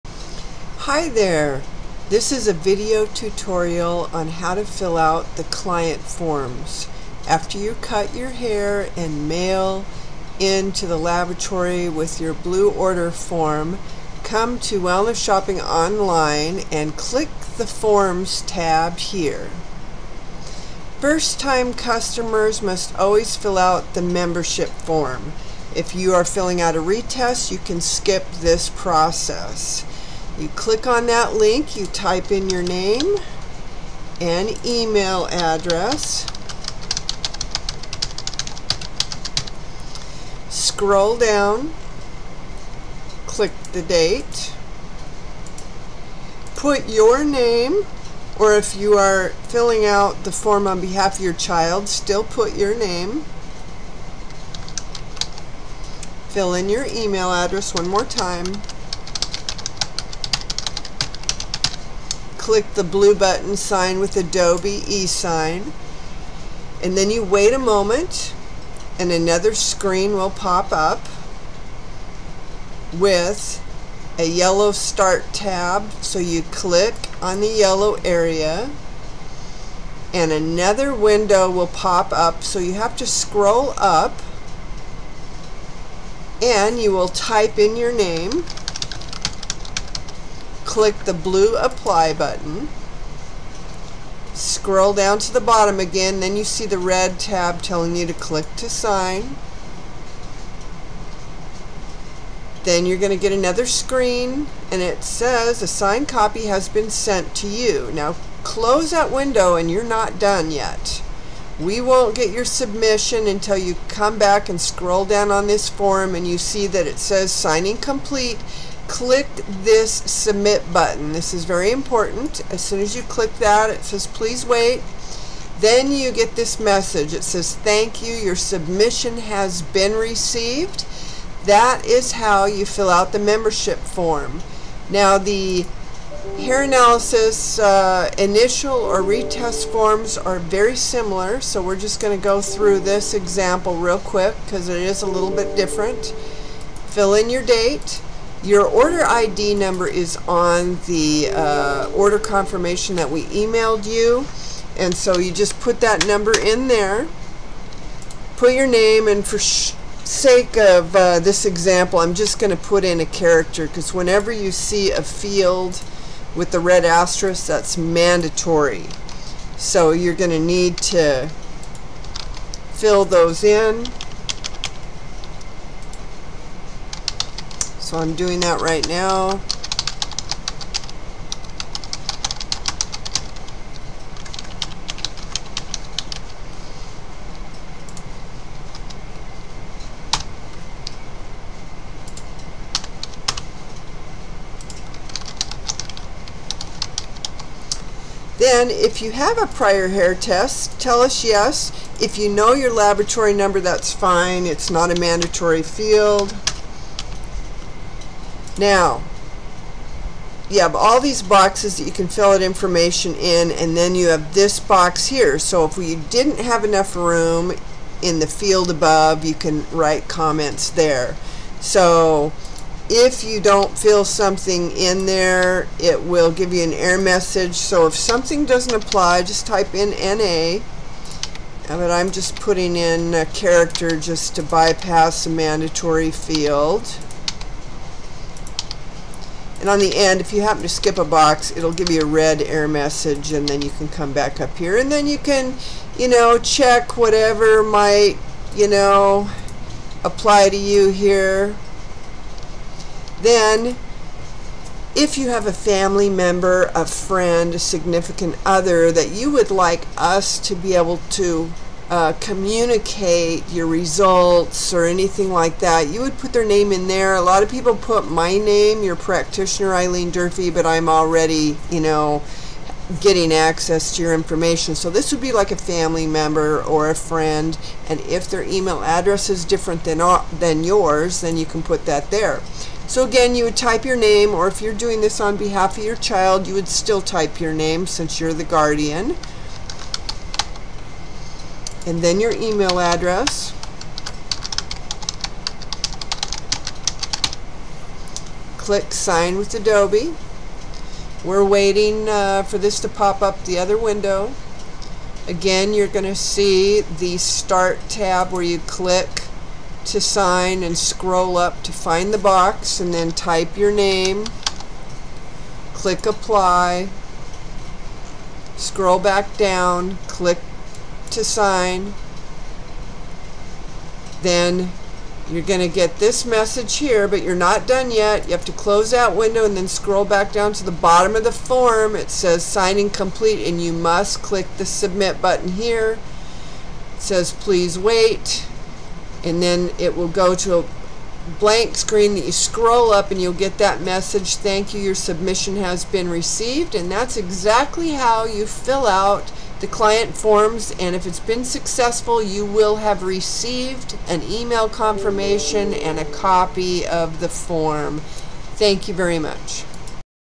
Client Hair Analysis Forms
Tutorial on how to fill out and sign the hair analysis client forms online.